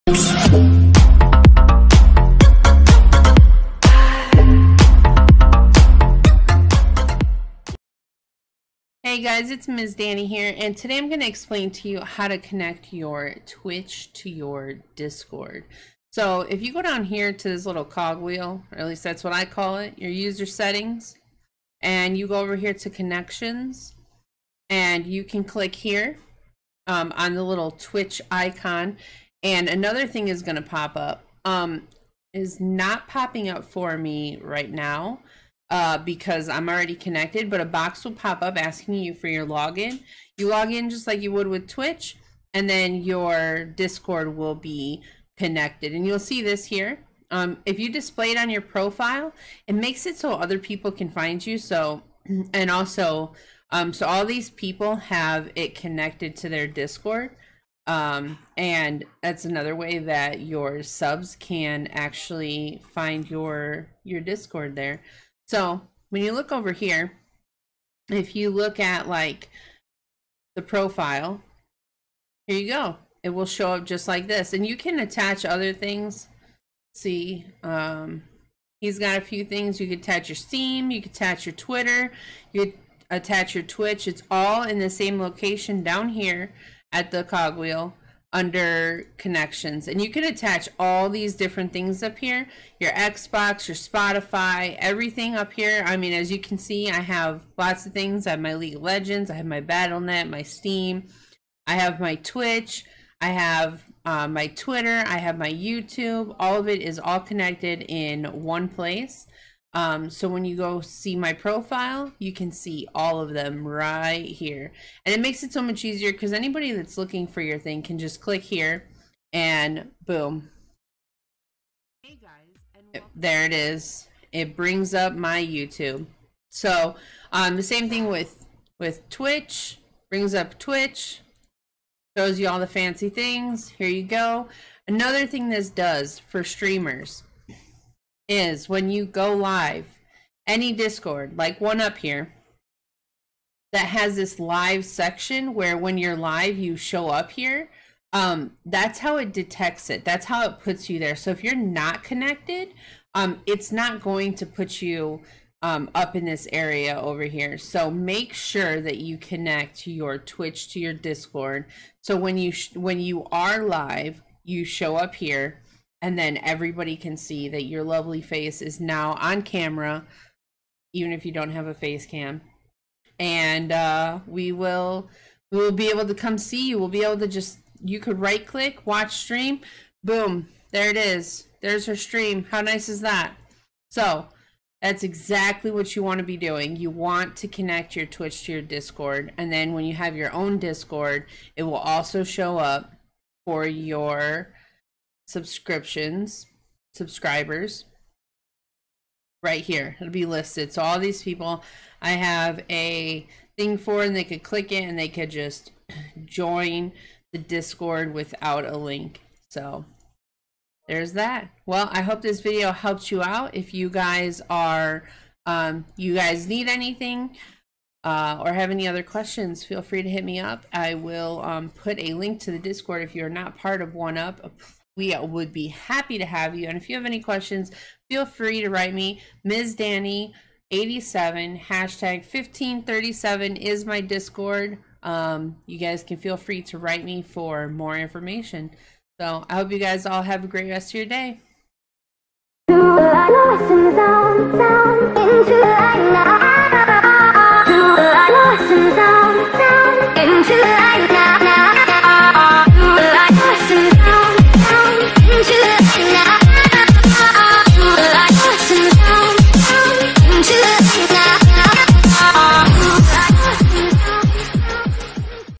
How to Connect Twitch and Discord (on Discord)!
This video teaches you how to connect your Twitch and your Discord. This will allow streamers to show up as live in the Now Live roles, and allows everyone to show up as subscribers in any discord that has this role setup!

Questions? Let me know!
Mornings: Mon- Thurs 11am Central